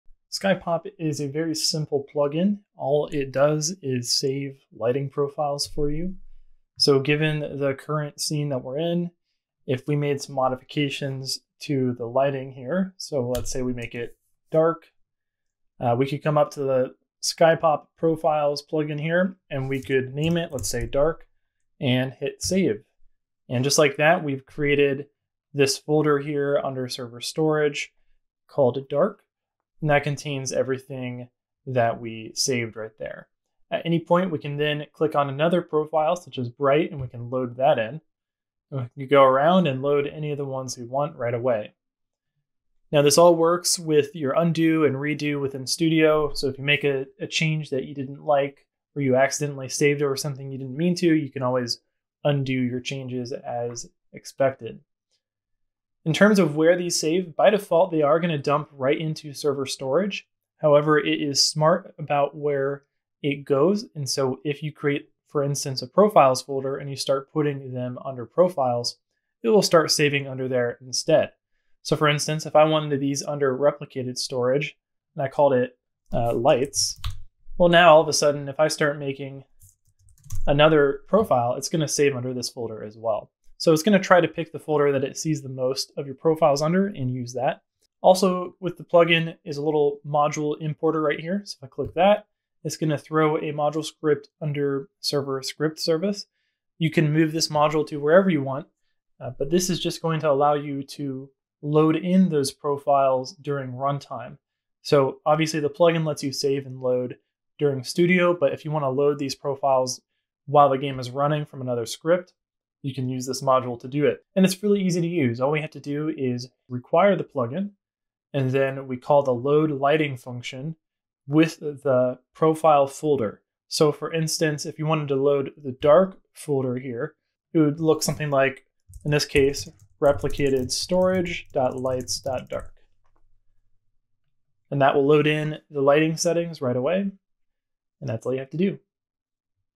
SkyPop Tutorial
Quick tutorial on using the SkyPop plugin.

0:00 Intro
0:06 Saving a Profile
0:33 Loading a Profile
0:42 Undo/Redo Support
0:55 Change Save Location
1:28 Inserting ModuleScript
1:54 Using the ModuleScript